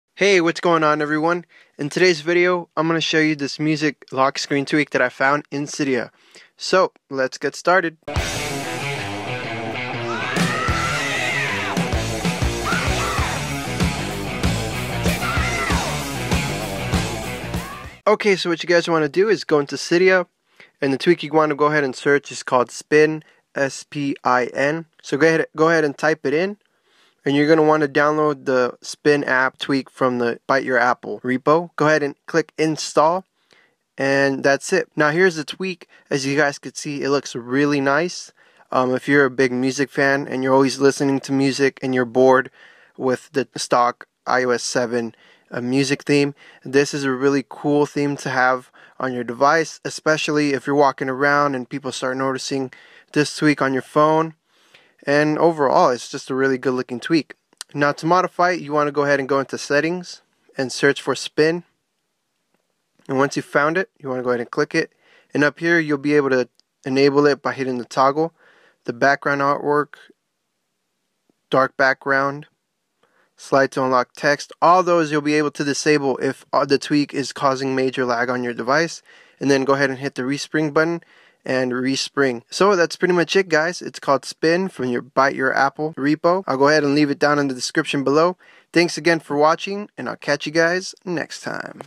MUST Have Lockscreen Music Player (Jailbreak Tweak) for iPhone, iPad, and iPod Touch!
In this video ill be showing you how to get this cool lockscreen music tweak from cydia.

Steps:

1.) Open Cydia

2.) Click on search

3.) Search for "spin" from the biteyourapple repo

4.) Click install

5.) Go into settings to configure

6. ) You're done!

Enjoy! :D